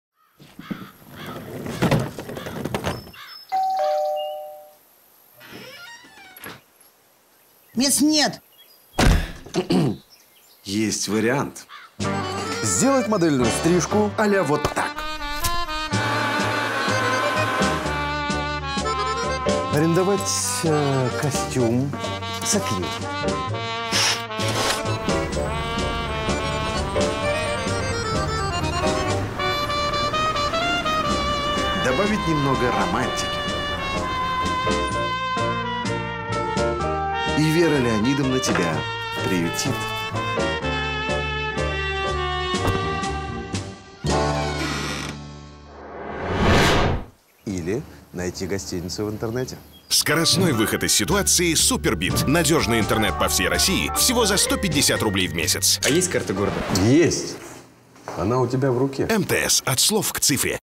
Мест нет, но есть вариант! (1 мин) | Опция «СуперБИТ» МТС
Дмитрий Нагиев знает выход из самых затруднительных ситуаций: скоростной интернет по всей России с опцией «СуперБИТ»